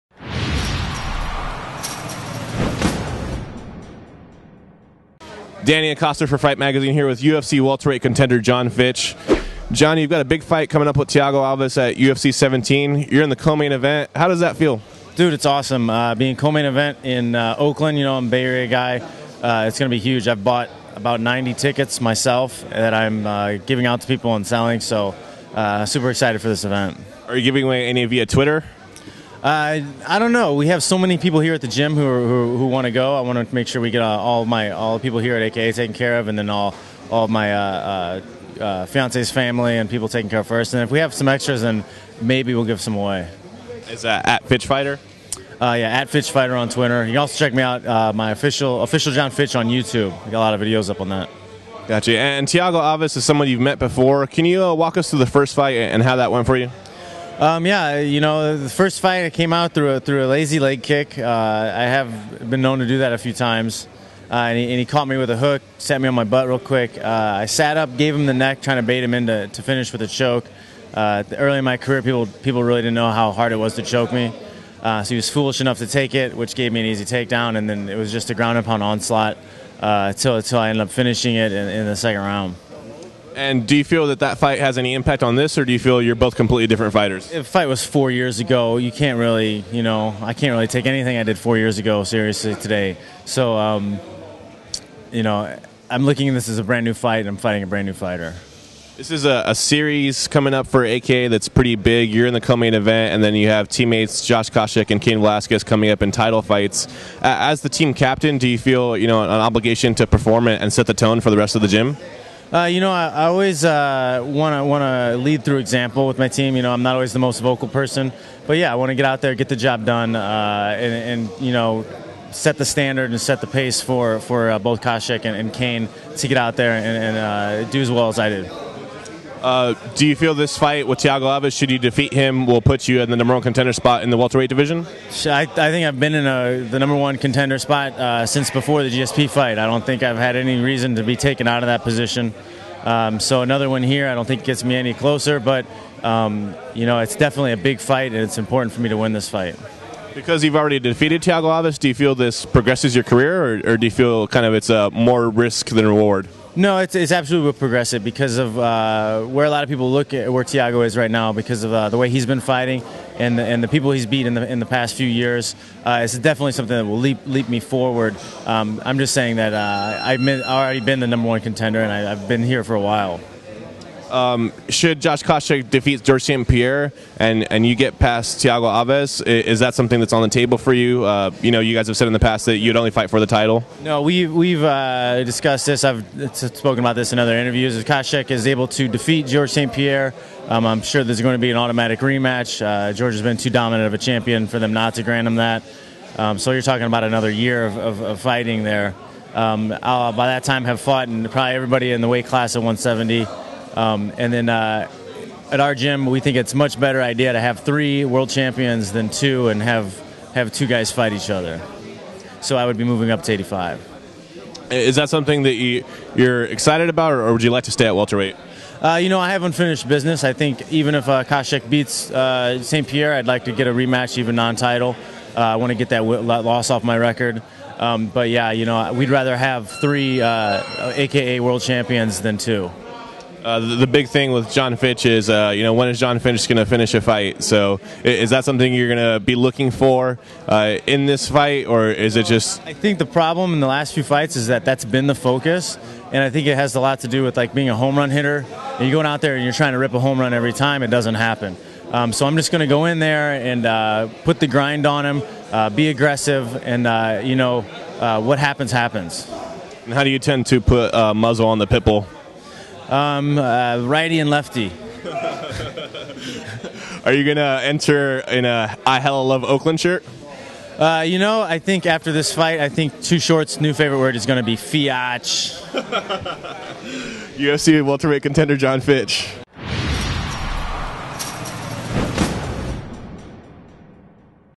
Jon Fitch: I'm Fighting a Brand New [Alves]
UFC welterweight contender Jon Fitch hopes to set the tone for the American Kickboxing Academy with a major win over fellow former title challenger Thiago Alves at UFC 117. Interview by Danny Acosta. Shot and edited by RIck Lee.